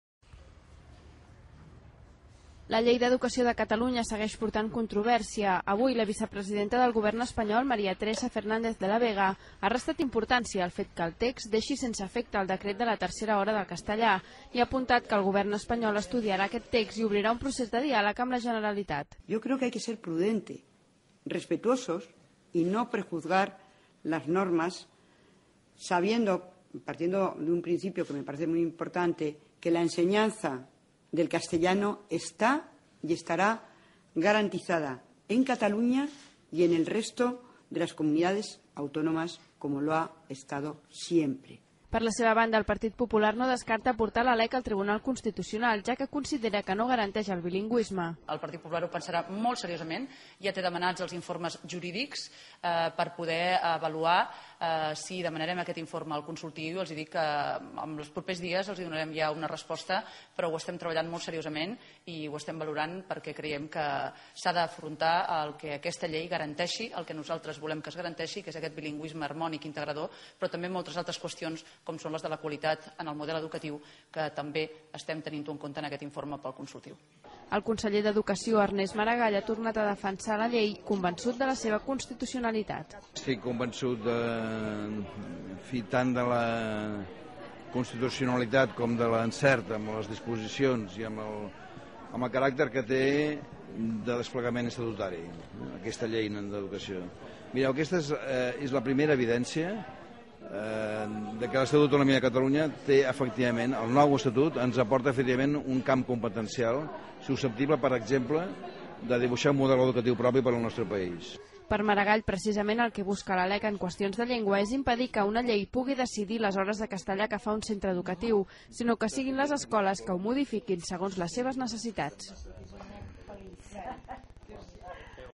De la Vega sobre la LEC: 'L'ensenyament del castellà està i estarà garantit a Catalunya'
MAdrid (ACN).- La vicepresidenta del govern espanyol, Maria Teresa Fernández de la Vega, ha restat transcendència aquest divendres a la controvèrsia sobre la nova Llei d'Educació de Catalunya aprovada en comissió, que deixa sense efecte el decret de la tercera hora de castellà. De la Vega ha demanat 'prudència' a l'hora de valorar-la i ha apuntat que el seu executiu estudiarà el text i obrirà un procés de diàleg amb la Generalitat. En tot cas ha volgut deixar clar que 'l'ensenyament del castellà està i estarà garantit a Catalunya i a la resta de les Comunitats Autònomes' i ha recordat que l'aprenentatge del castellà 'a vegades fins i tot és més elevat en els territoris que tenen llengües cooficials'.